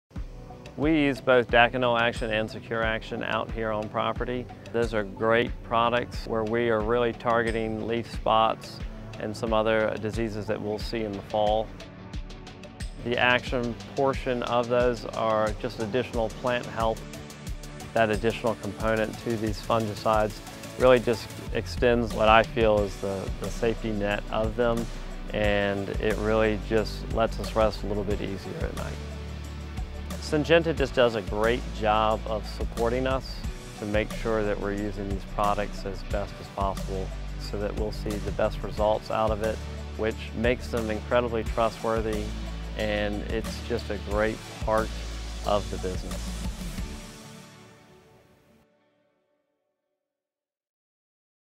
Daconil Action: Take Action Against Leaf Spots
Robert Kilduff, director of agronomy at Century Golf in Orlando, Florida, discusses how they use Daconil® Action™ fungicide to prevent leaf spots and other diseases they may encounter in the fall. With support from Syngenta, they can use their products right and see great results. for more information.